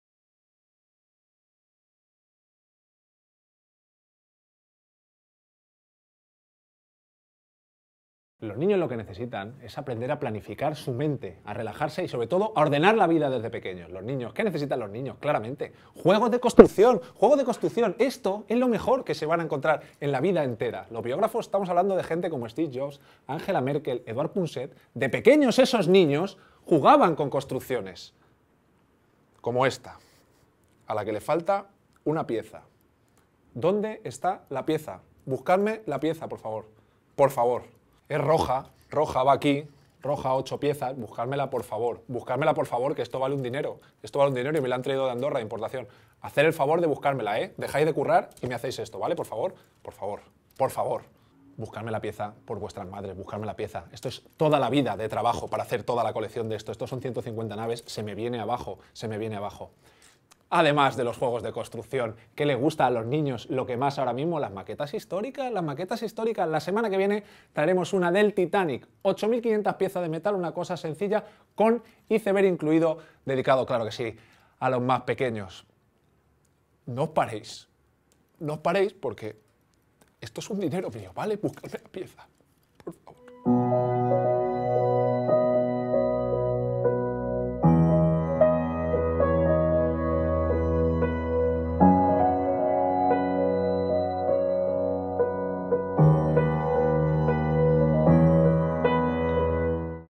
Qué necesitan los niños - Quique Peinado - Acción contra el Hambre
Desde hace años, los expertos intentan averiguar qué necesitan los niños realmente.

Esta vez es Emilio Aragón, Vicepresidente de Acción contra el Hambre, quien hace esta pregunta a personas no menos importantes…

Sus respuestas fueron… Bueno será mejor que lo veas por ti mismo.

y mira el resto de vídeos.